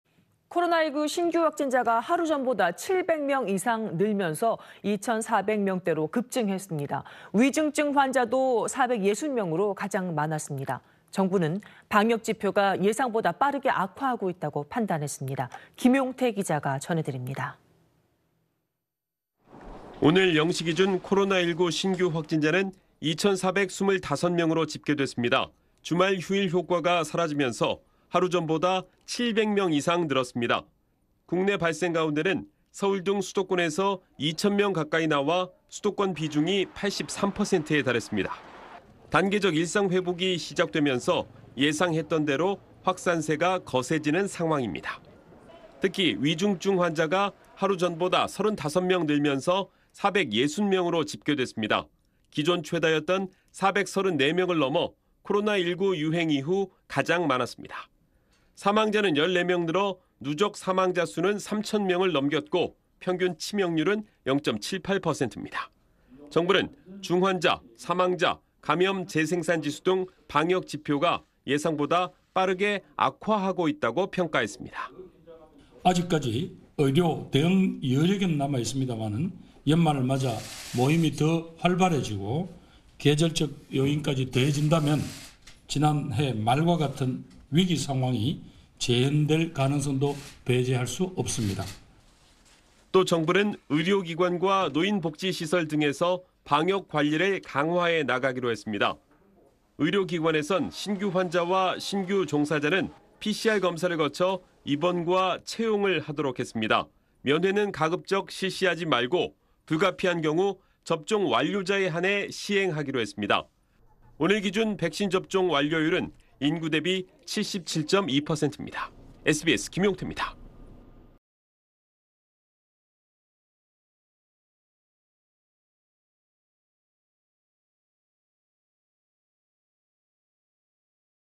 위중증 역대 최다…총리 "방역 지표 예상보다 빠르게 악화" / SBS
코로나19 신규 확진자가 하루 전보다 7백 명 이상 늘면서 2천4백 명대로 급증했습니다. 위중증 환자도 460명으로 가장 많았습니다. 정부는 방역 지표가 예상보다 빠르게 악화하고 있다고 판단했습니다.
김용태 기자가 전해드립니다.